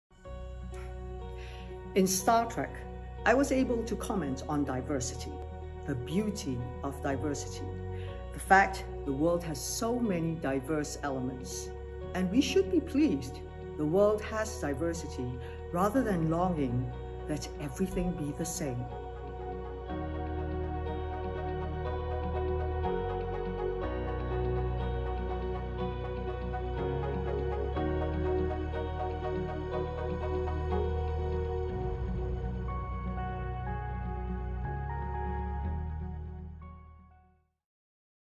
Michelle Yeoh Quoting Gene Roddenberry: "The Beauty of Diversity"
We're sharing 100 of Star Trek creator Gene Roddenberry’s most impactful quotes over the course of the 100 days leading up to his 100th birthday on August 19, 2021!

"In Star Trek, I was able to comment on diversity, the beauty of diversity, the fact the world has so many diverse elements, and we should be pleased that the world has diversity rather than longing that everything be the same." — Gene Roddenberry